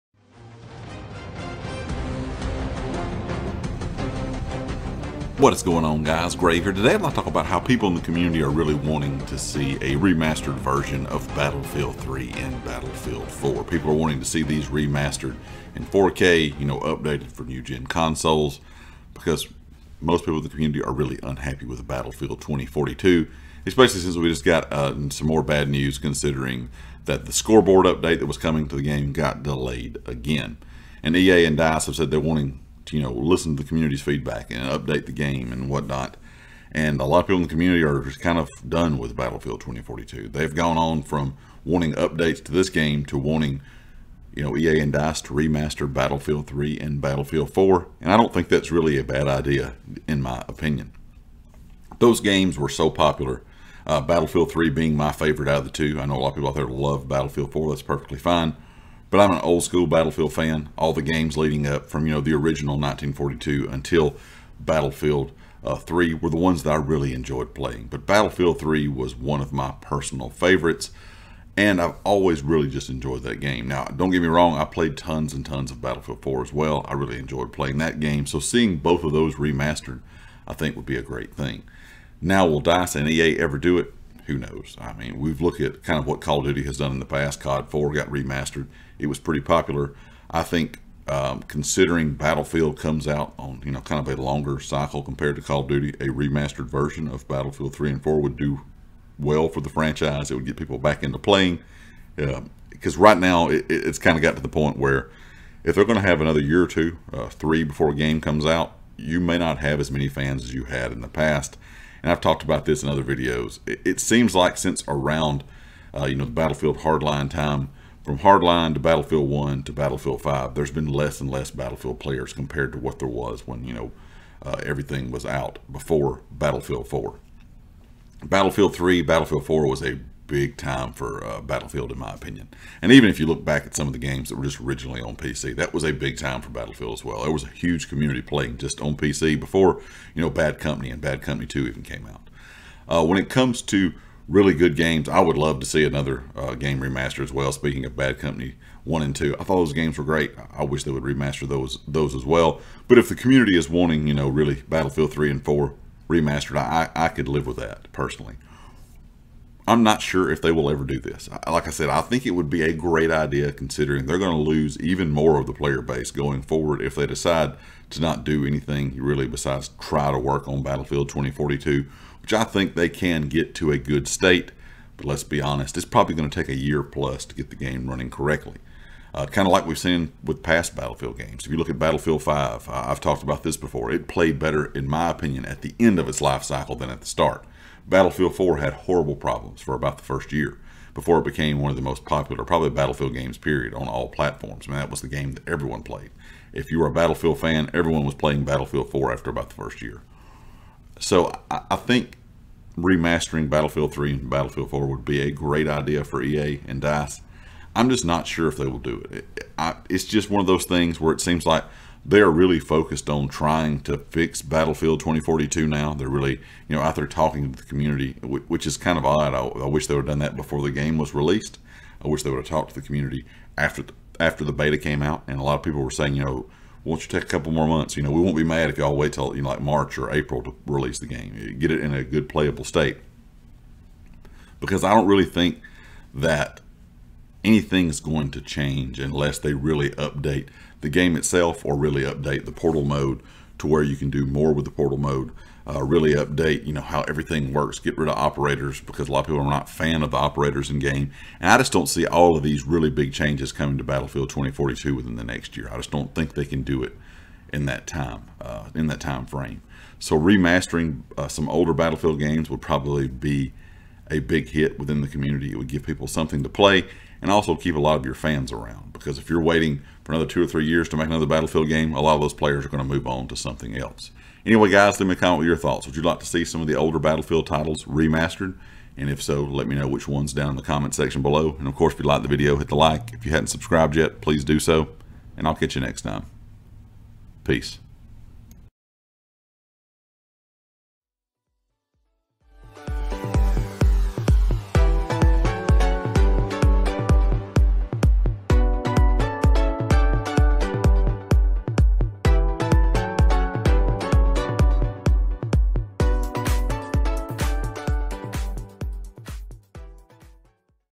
Community Wants EA To Remaster Battlefield 3 and 4
The community wants EA and dice to remaster battlefield 3 and 4.